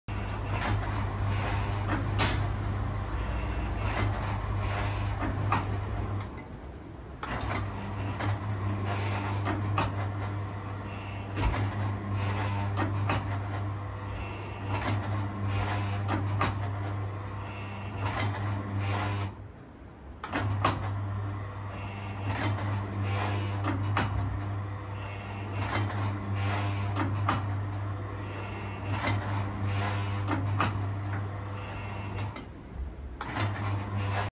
Clay Pigeon Trap for Sale DTL mode
Farey 400 clay pigeon capacity Automatic Ball Trap (ABT) in Down the LIne (DTL) mode.

Timer can be adjusted so the machine can run continuously or stop sooner.